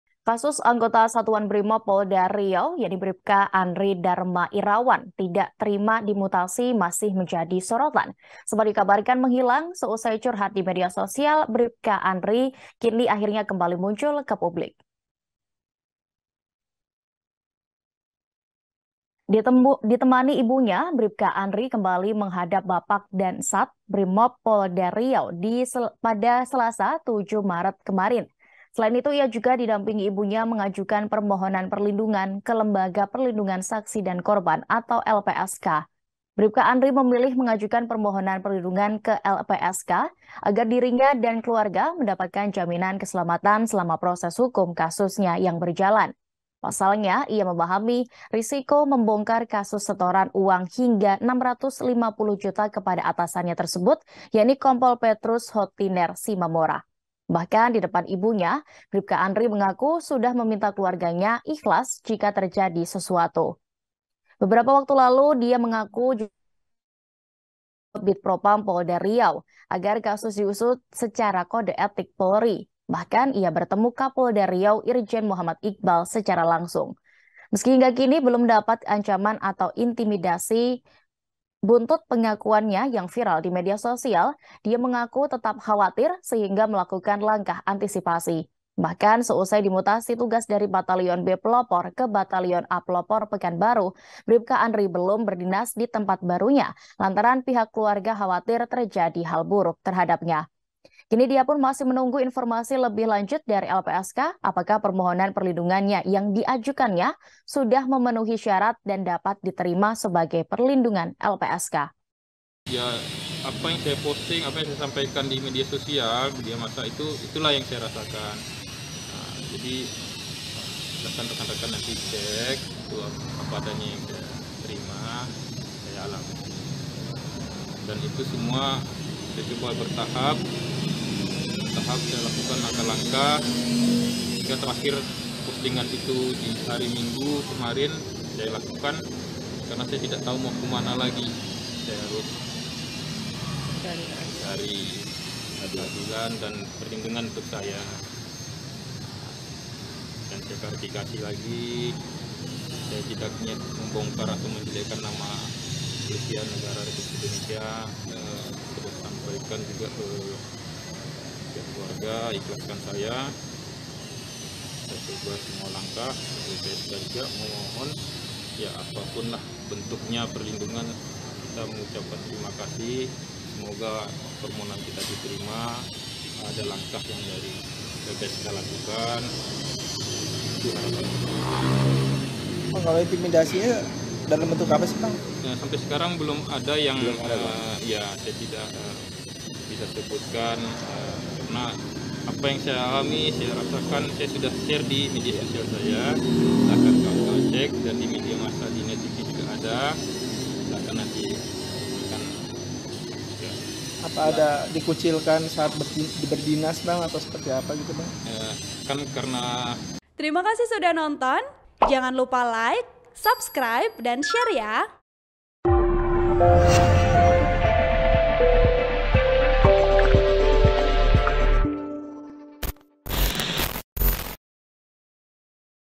Bripka Andry Muncul Perdana seusai Menghilang Pasca-Bongkar Bobrok Komandan Brimob Polda Riau
TRIBUN-VIDEO.COM - Kasus anggota Satuan Brimob Polda Riau, Bripka Andry Darmairawan tak terima dimutasi masih menjadi sorotan.

Ditemani ibunya, Bripka Andry kembali menghadap Bapak Dansat Brimob Polda Riau di hari Selasa tanggal 7 Maret 2023.

Selain itu, ia juga didampingi ibunya mengajukan permohonan perlindungan ke Lembaga Perlindungan Saksi dan Korban (LPSK).

Bripka Andry memilih mengajukan permohonan perlindungan ke LPSK agar dirinya dan keluarga mendapat jaminan keselamatan selama proses hukum kasus berjalan.

Pasalnya, ia memahami risiko membongkar kasus setoran uang Rp650 juta kepada atasannya Kompol Petrus Hottiner Simamora.

Bahkan di depan ibundanya, Bripka Andry mengaku sudah meminta keluarganya ikhlas jika terjadi sesuatu.

Beberapa waktu lalu dia sudah melaporkan kasus ke Bidpropam Polda Riau agar kasus diusut secara kode etik Polri.

Bahkan bertemu Kapolda Riau Irjen Mohammad Iqbal.

Bahkan usai dimutasi tugas dari Batalyon B Pelopor ke Batalyon A Pelopor Pekanbaru, Bripka Andry belum berdinas di tempat barunya lantaran pihak keluarga khawatir terjadi hal buruk.

Kini dia masih menunggu informasi lebih lanjut dari LPSK apakah permohonan perlindungan diajukannya sudah memenuhi syarat, dan dapat diterima sebagai terlindung LPSK.(Tribun-video.com)